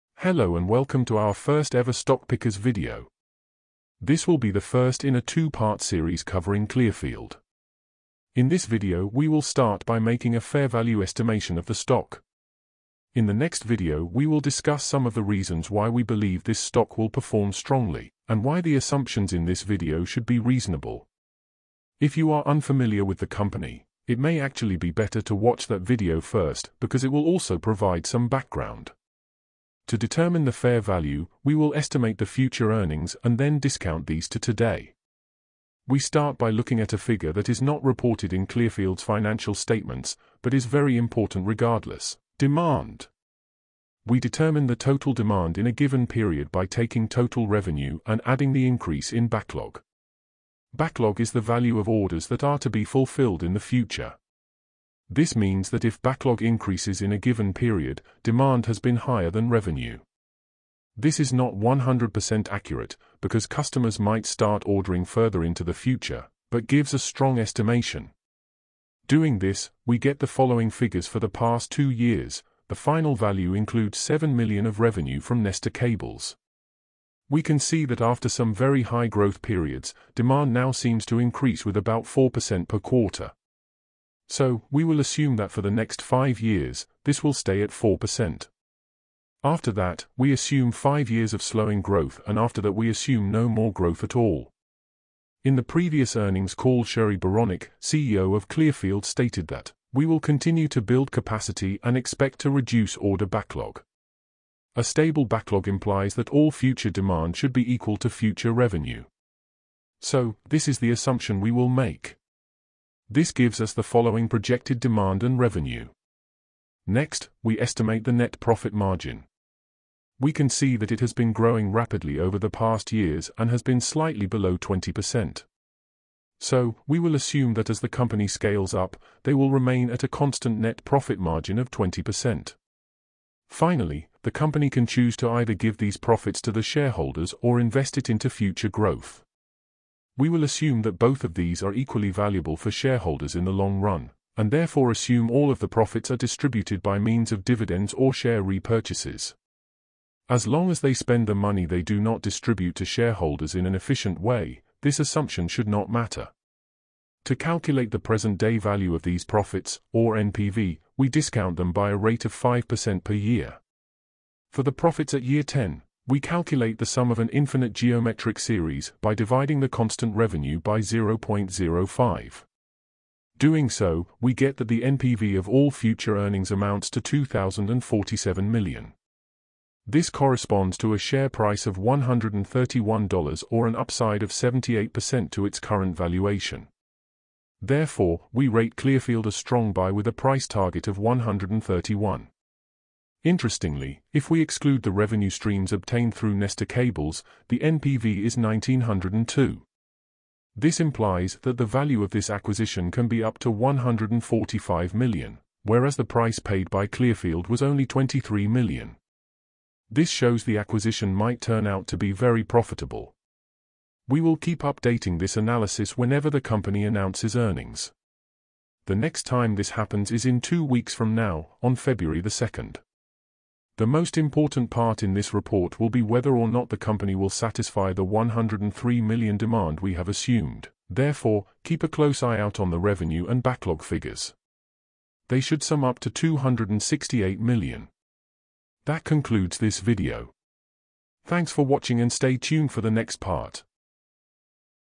Why Clearfield (Clfd) should be $131 (not $73)
This video shows why the fair value of Clearfield (Clfd) would put it at $131. Hence, we rate it a strong buy at its current share price of $73.